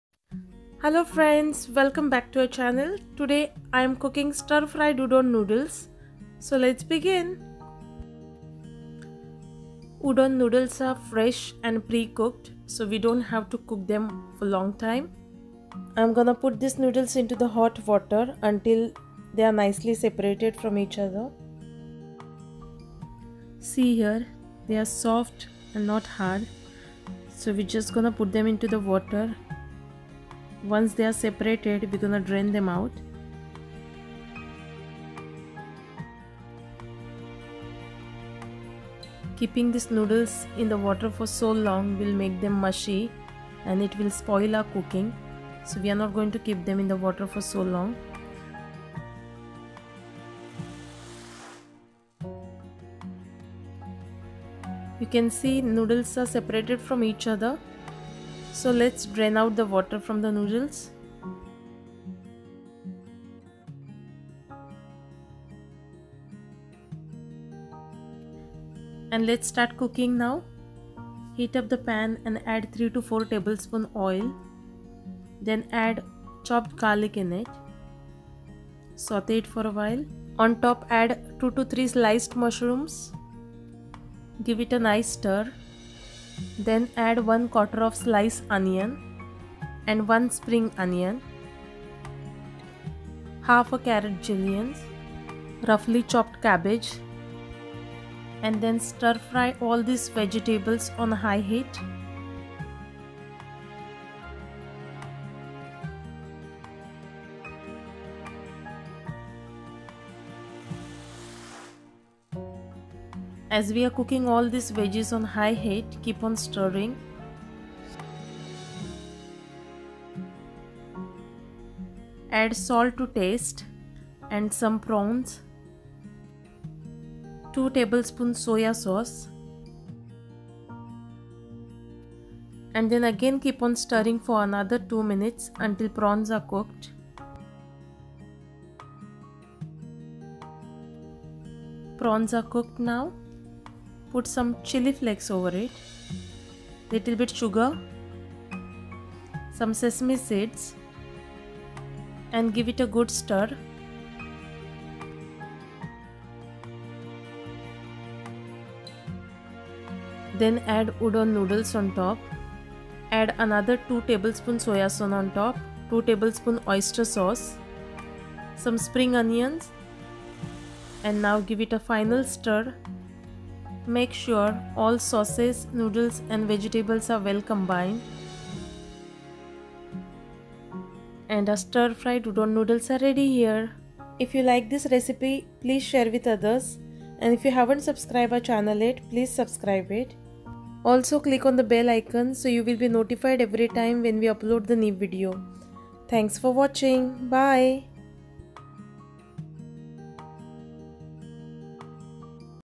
Udon Noodles Recipe | Stir Fry Udon Noodles | How to make Stir Fried Udon Noodles
Udon is a type of thick, wheat-flour noodles used frequently in Japanese cuisine. These noodles are stir fried along with verity of veggies and meat or shrimps and sauces.